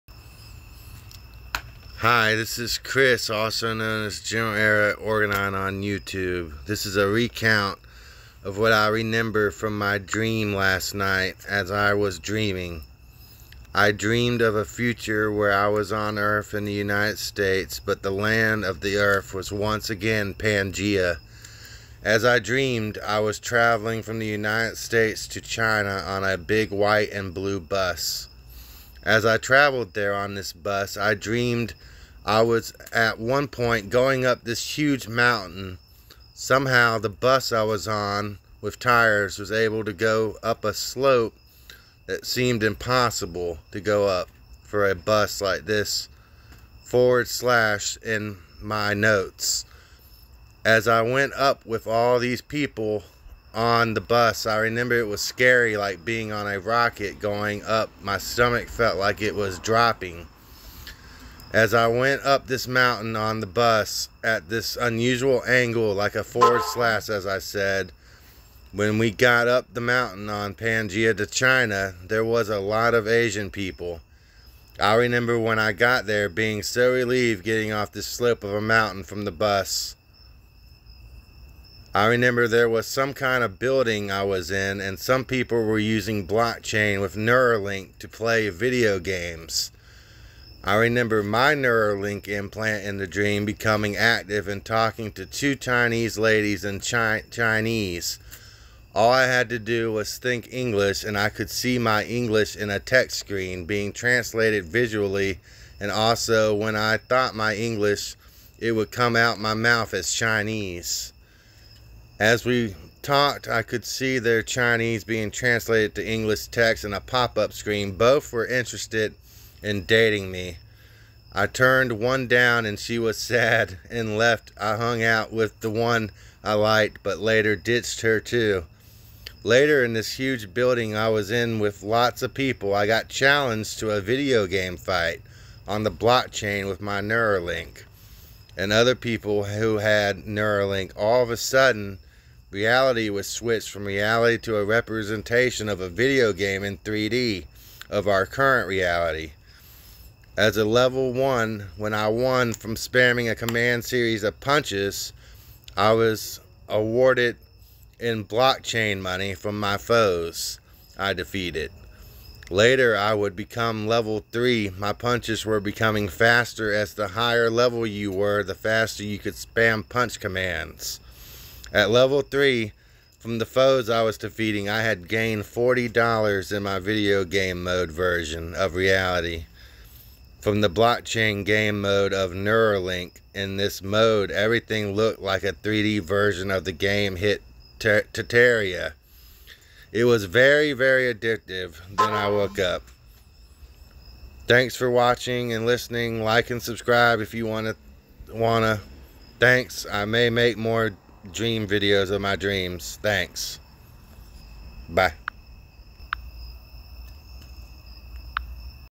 A dream of a future trip through pangea to china with Nero link from the United states
A dream of a future trip through pangea to china 🇨🇳 with Nero link implant to pangea from the United states 🇺🇲 enough said.